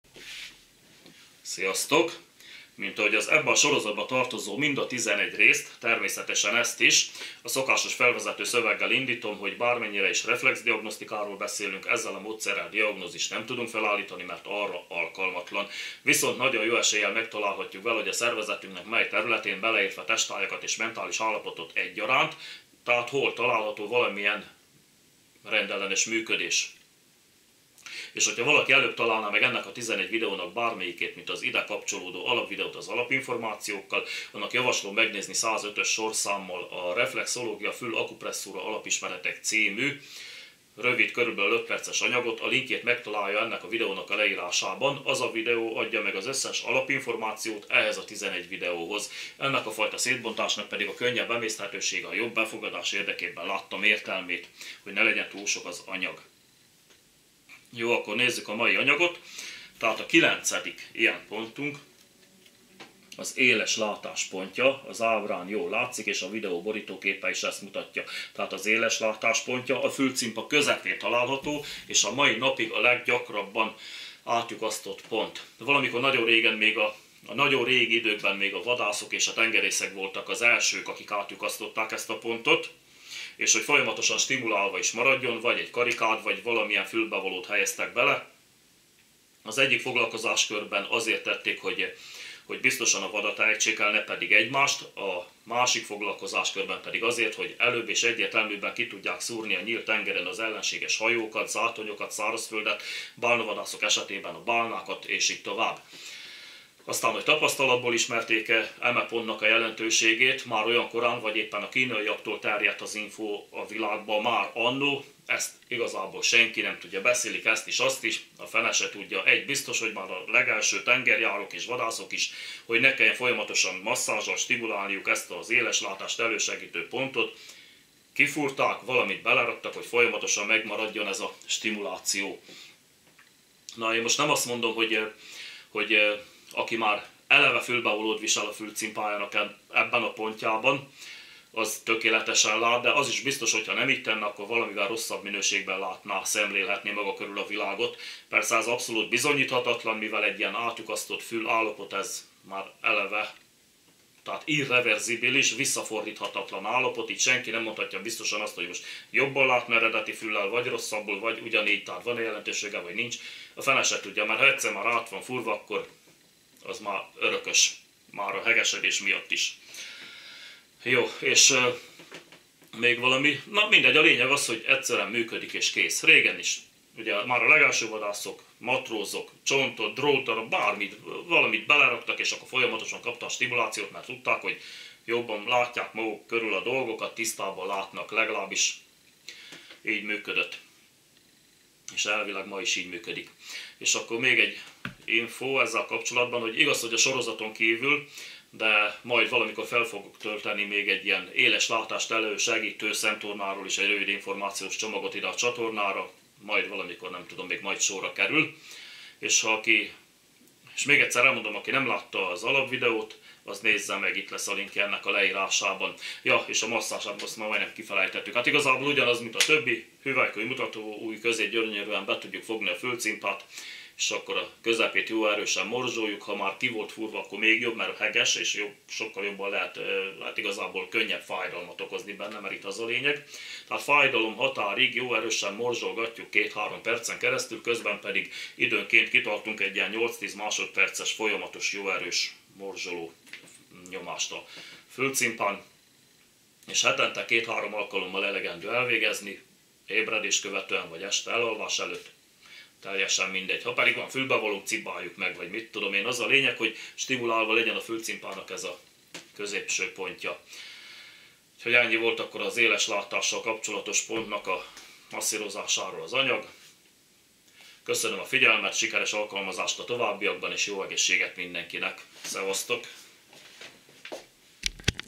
Fül akupresszúra 9.: Éleslátás pontja
A látásjavító pont masszírozásával kiváltható az örökös stimulációt szolgáltató fülékszerek viselése, melyek eredetileg is az élesebb látást voltak hivatottak segíteni. A történelem első vadászai és tengerészei voltak az elsők, akik átfúrták az említett ponton a fülcimpájukat a hatékonyabb kémlelés és célzás érdekében.